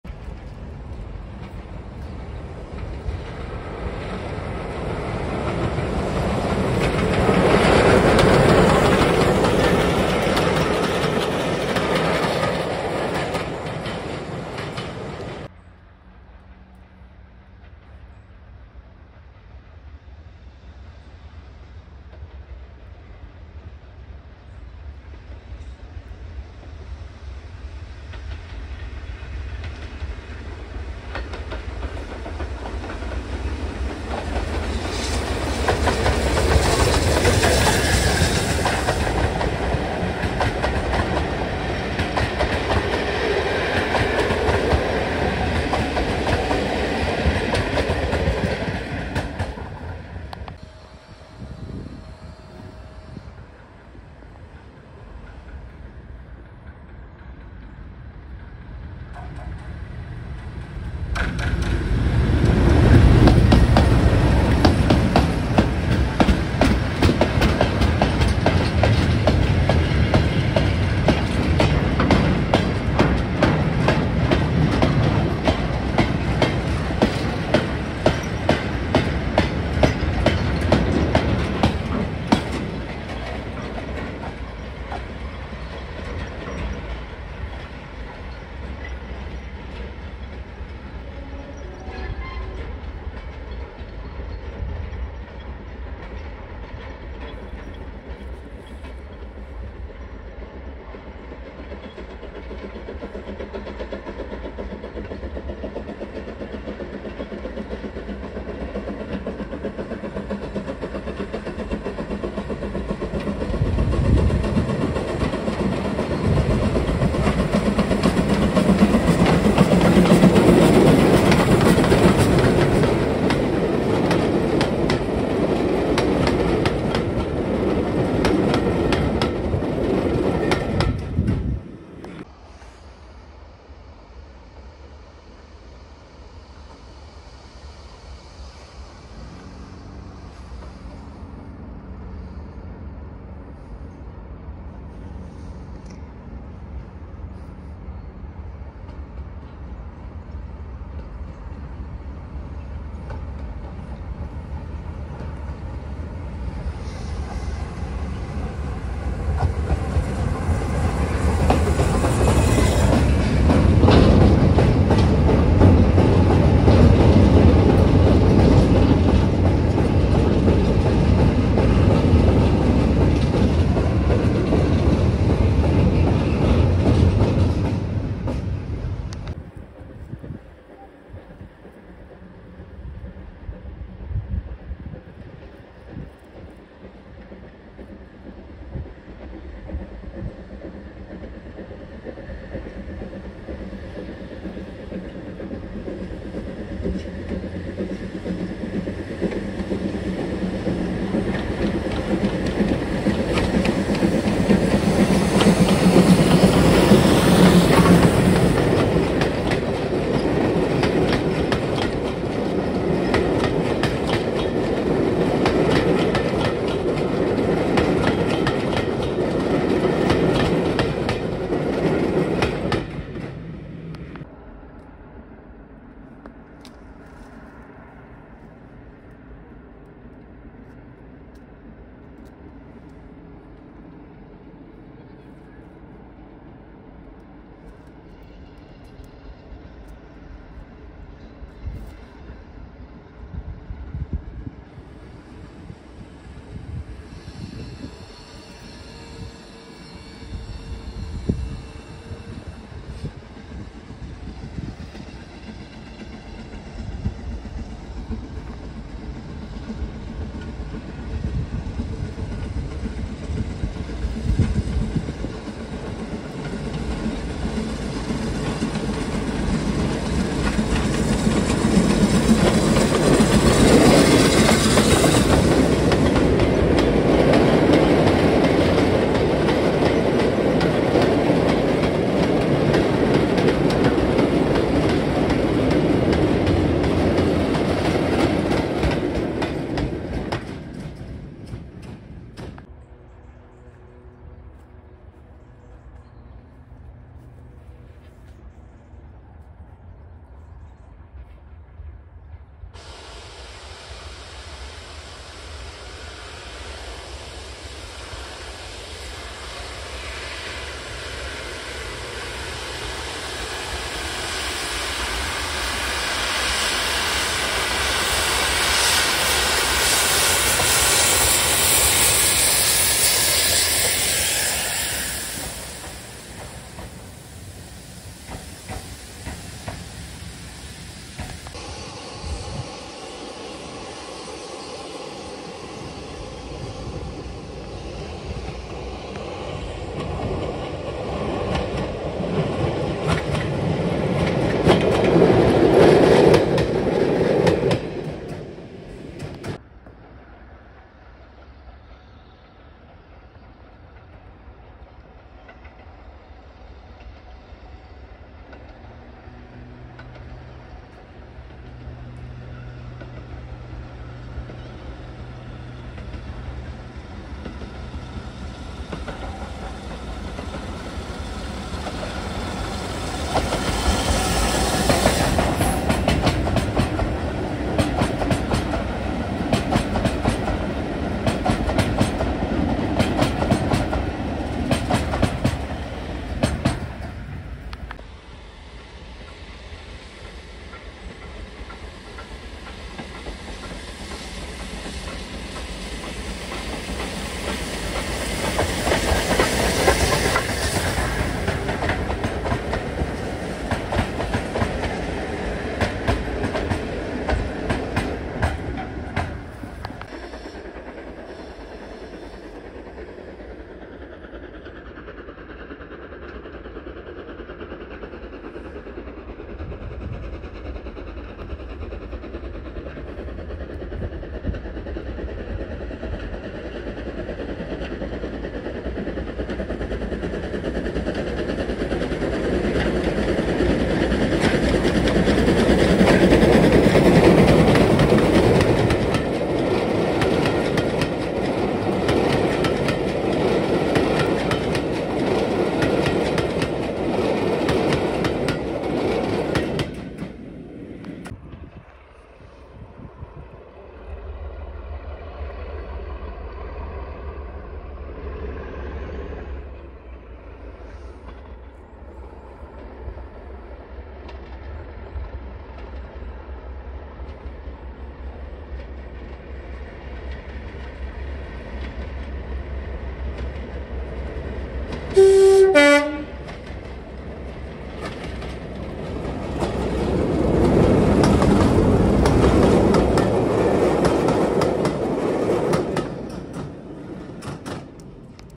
Great Central Railway Autumn Steam Gala 2022
Filmed at Daffodil Close
Locos
Class 37 D6700
LMS Class 8 48305
Ivatt 2 46521
Ivatt 2 tank 41312
J27 65894